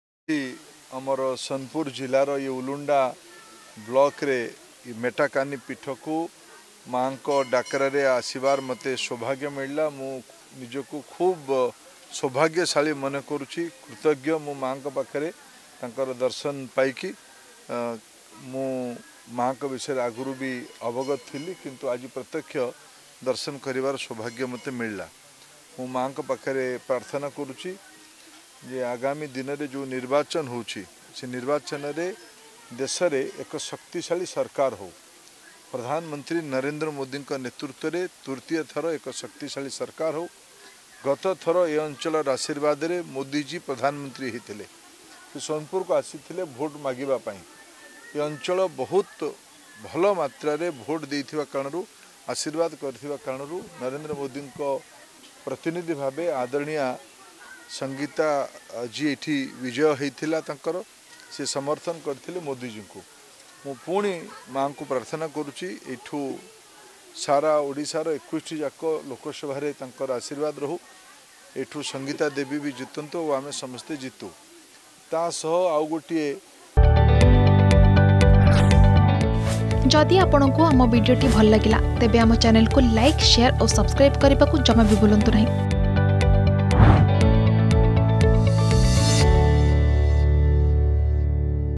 ସୋନପୁରରୁ ସଂଗୀତା କୁମାରୀ ସିଂଦେଓଙ୍କ ବିଜୟ କାମନା କରୁଛି : ଧର୍ମେନ୍ଦ୍ର ପ୍ରଧାନ || Dharmendra Pradhan
ସୋନପୁର ଉଲୁଣ୍ଡା କୁଦମେଣ୍ଡାରେ ମା' ମେଟକାନୀ ମନ୍ଦିରରେ ଦର୍ଶନ କରିବା ସହ ବିଜେପି ସାଂସଦ ପ୍ରାର୍ଥୀ ସଂଗୀତା କୁମାରୀ ସିଂଦେଓଙ୍କ ବିଜୟ କାମନା କଲେ କେନ୍ଦ୍ରମନ୍ତ୍ରୀ ଧର୍ମେନ୍ଦ୍ର ପ୍ରଧାନ ।